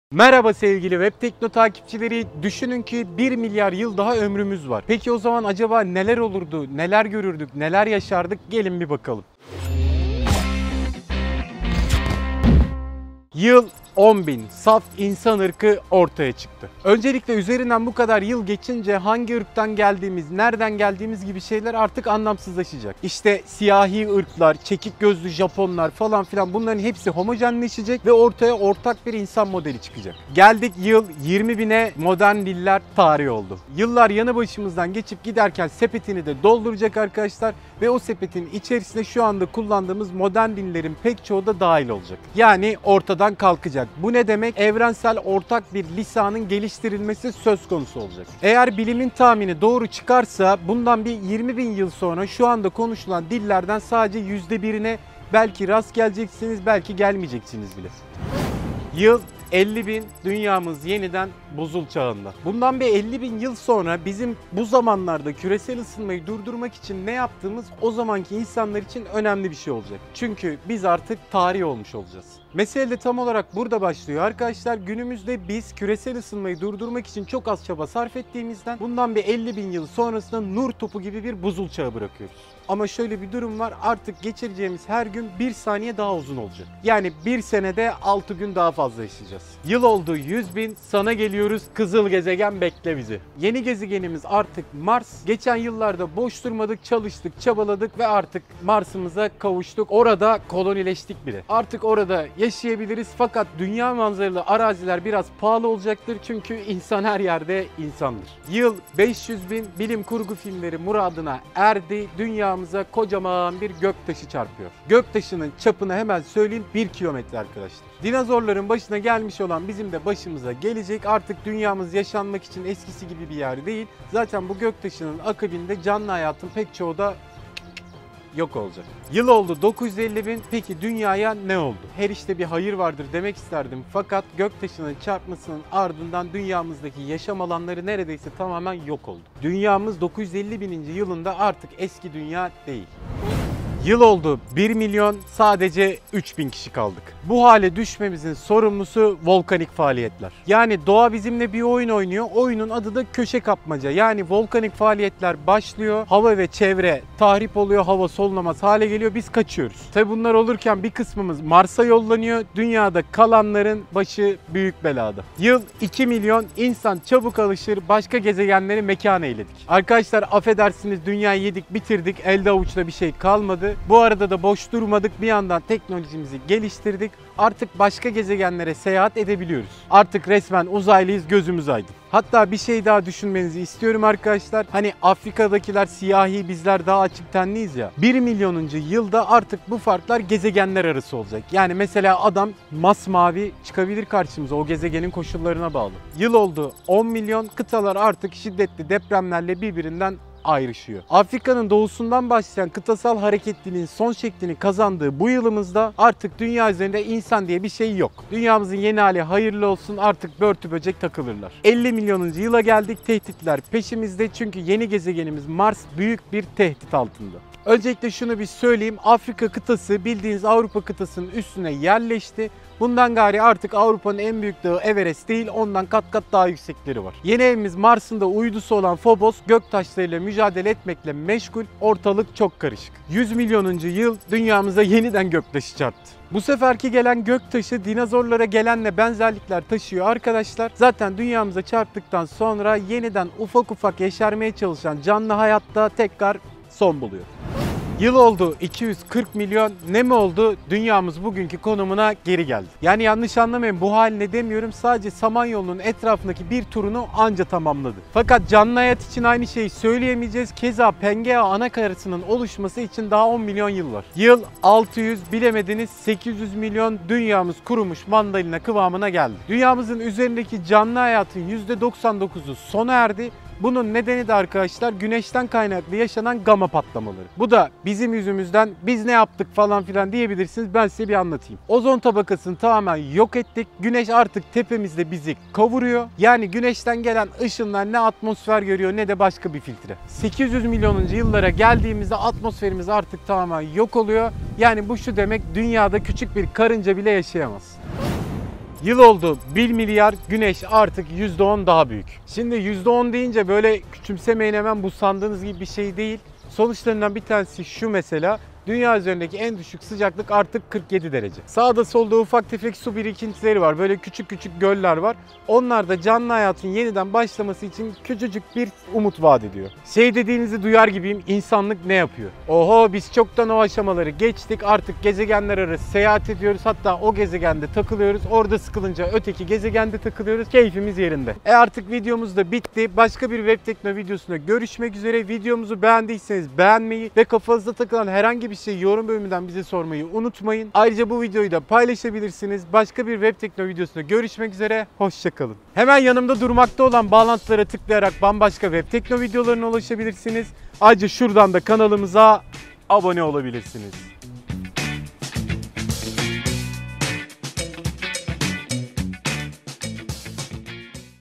Eğer 1 Milyar Yıl Daha Ömrümüz Olsaydı Nelere Tanık Olurduk?
Merhaba sevgili Webtekno takipçileri. Düşünün ki 1 milyar yıl daha ömrümüz var. Peki acaba o uzun yıllar boyunca nelere tanık olurduk? Gelin hep beraber bi bakalım, 1 milyar yıl yaşarsak neler olurmuş.

Direkt konumuza geçelim arkadaşlar:

Yıl 10.000: Saf İnsan Irkı Ortaya Çıktı!

Öncelikle arkadaşlar üzerinden binlerce yıl geçince nereden geldiğimiz, hangi ırkın mensubumuz gibi şeyler anlamsızlaşacak. En azından homojenleşecek. Yani siyahi insanmış, çekik gözlü insanmış falan filan gibi şeyler ortadan kalkacak. 10.000 yıl sonrası için şunu söyleyebiliriz belki de “saf insan ırkı ortaya çıkmış olacak.”

Yıl 20.000: Modern Diller Tarih Olur!

Yıllar geçerken sepetini dolduracak arkadaşlar maalesef. Bu sepetin içerisinde günümüzde kullandığımız farklı farklı modern dillerin ortadan kalkması da var. Eğer bilimin tahmini doğru çıkarsa 20.000 yıl sonrasında bugün konuşulan dillerden sadece %1’i varlığını koruyabilecek.

Yıl 50.000: Dünya Yeniden Buzul Çağında

50.000 yıl sonra küresel ısınmayı durdurmak için ne yaptığımızı merak edecek insanlar olacaktır. Çünkü biz artık tarih olacağız. Mesele de burada başlıyor; küresel ısınmayı durdurmak için çok az çaba sarfettiğimizden 50.000 yıl sonrasına nur topu gibi bir buzul çağı bırakıyoruz.

Yıl 100.000: Sana Geliyoruz Kızıl Gezegen!

Bu ne demek? Evet, doğru tahmin. Pılı pırtı toplayın Mars’a taşınıyoruz. Yeni evimiz artık Mars. Geçen 100.000 yıl boyunca hiç boş durmadık hep çalıştık ve Mars’a gide gele Mars’ı da dünya’ya benzettik. Artık orada yaşayabiliriz, dünya manzaraları arazilerin fiyatları uçmuş durumda.

Yıl 500.000: Bilim Kurgu Muradına Erdi: Dünyamız Dev Bir Astreoit İle Çarpışır!

1 Kilometre çapındaki bir astreoit ile çarpışacağız. Dinozorların başına gelen bizim de başımıza gelmiş olacak. Dünya üzerinde yaşam artık bildiğiniz gibi değil.

Yıl 950.000: Dünyaya Ne Oldu?

Maalesef “her işte bir hayır vardır” oldu. Yani dünyamız yaşanan felaket ve gökyaşı çarpması sonucu tamamen yerle bir oldu. Dünya artık o eski bildiğimiz dünya değil.

Yıl 1 Milyon: Sadece 3.000 Kişi Kaldık!

Sadece 3.000 kişiyiz, bunun nedeni gittikçe şiddetlenen volkanik faaliyetler yer yüzünde bize doğayla bir oyun oynatıyor. O oyunun adı: köşe kapmaca.

Yıl 2 Milyon: İnsan Alışır: Başka Gezegenleri Mekan Eyledik!

Arkadaşlar affedersiniz Dünya’yı yedik bitirdik harbiden elde avuçta bir şey kalmadı yeni teknolojimiz sayesinde gezegenler arası rahatça gezebiliyoruz ve bize uygun yerlerde kolonileşebiliyoruz. Resmen uzaylıyız, gözümüz aydın.

Yıl 10 Milyon: Kıtalar Şiddetli Depremlerle Ayrılmaya Başladı

Şimdi bu aşamada dünya üzerinde insan kalmamış oluyor. Afrika’’nın doğusundan başlayan kıtasal hareketlilik artık son şeklini kazandı. Dünyamızın yeni hali hayırlı olsun, fakat üstünde biz yokuz.

Yıl 50 Milyon: Yeni Evimiz Mars Tehdit Altında!

Önce şunu söyleyeyim şiddetli depremler sonucunda Afrika, Avrupa kıtasının üstüne yerleşti. Bundan gari Avrupa’da Everest’ten yüksek dağlar var.

--Devamı için videomuzu izleyin!--